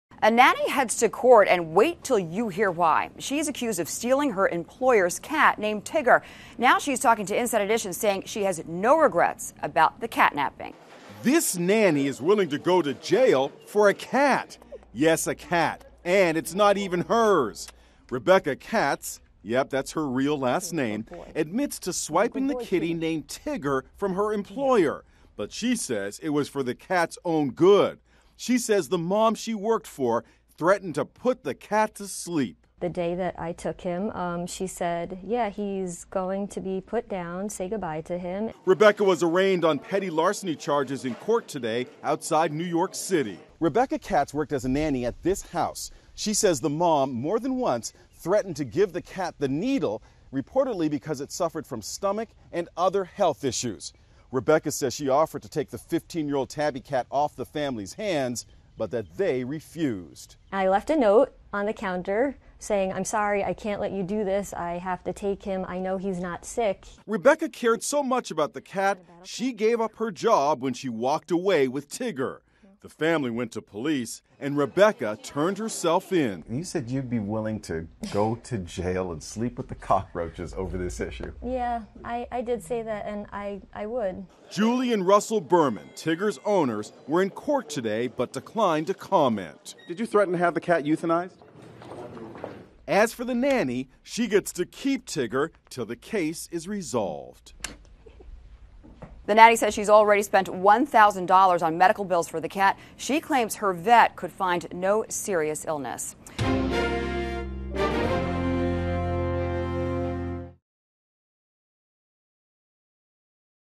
Ex-Nanny Faces Charges for Allegedly Stealing Family Cat
This New York nanny is willing to go to jail for a cat, and it's not even hers. Rebecca Katz faces petty larceny charges after she swiped her former Long Island employers' cat, Tigger, claiming that the family had threatened to put the 15-year-old animal to sleep. Katz, 33, said the mother of the family more than once said it was time to give the feline "the needle" because he had stomach and other health issues. Tigger's owners declined to comment.